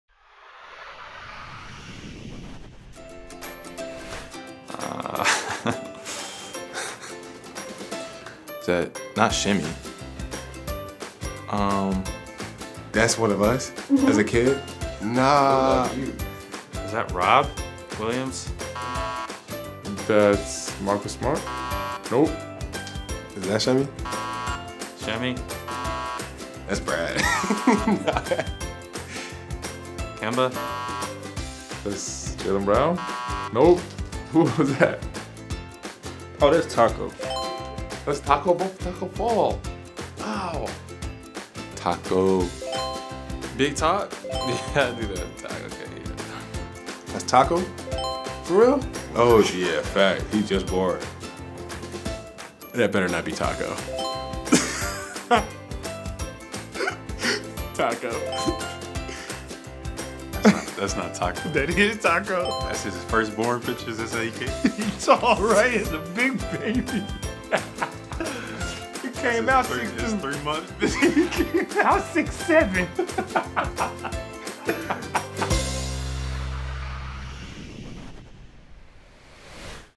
Guess That Baby Celtics Part 3 | Wingin' It with JetBlue
It's all love for Tacko no matter what age. Watch as Celtics players react to seeing Tacko Fall's baby picture.